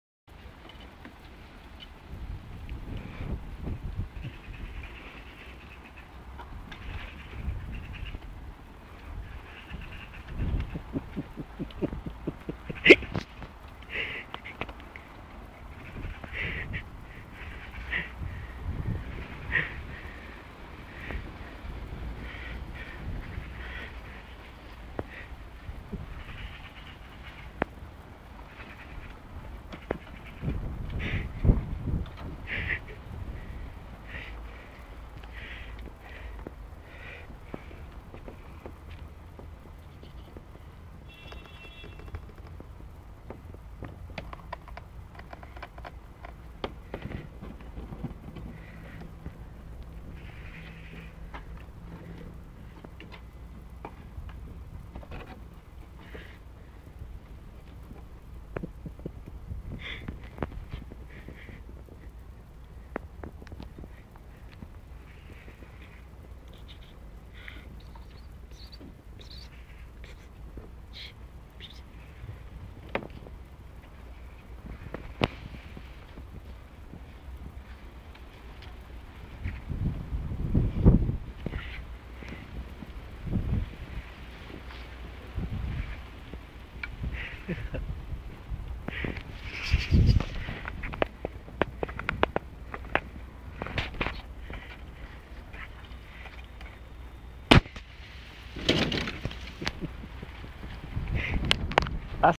Pussy Cat wants be a bird
This is our neighbours cat, which we call "purrdie" but I thnk it wants to be called "Birdie"

Tinker is the cat below trying to drag Purdie from it's perch!!